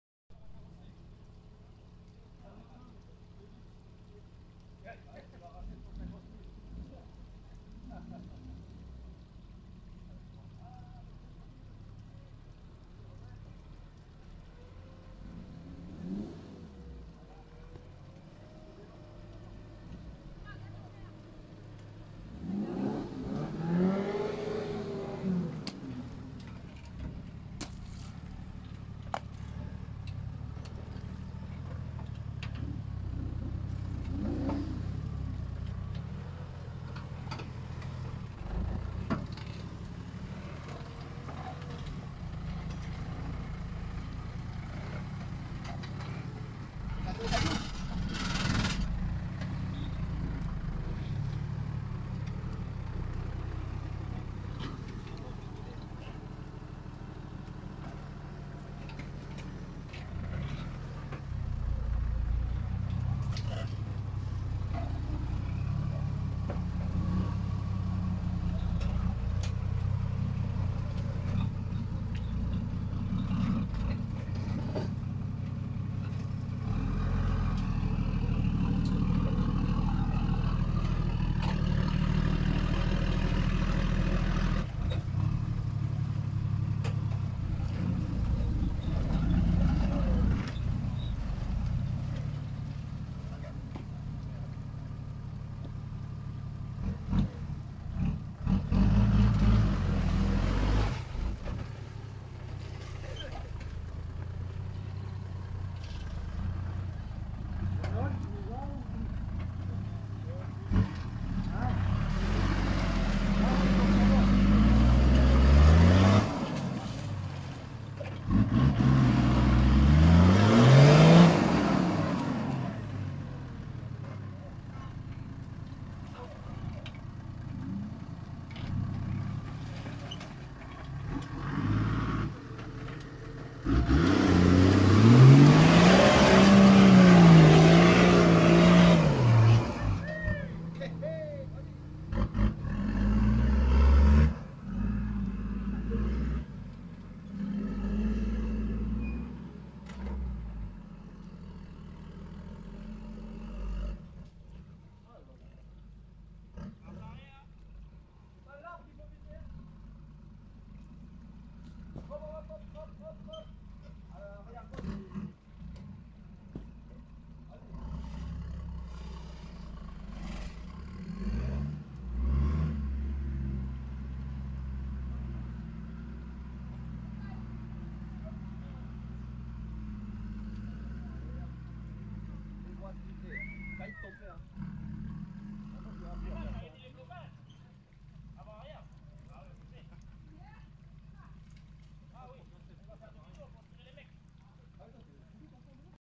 HDJ80 land cruiser toyota pointe de l abattoir 2022
Sortie avec le club double pont calédonien dans le sud.